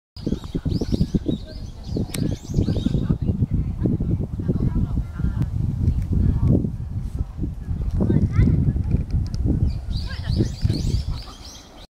五分咲きかな？東新田公園2014年3月28日午前12時半頃
木によってばらつきがありますが、五分咲きに近づいております。もうすぐ満開＼(^o^)／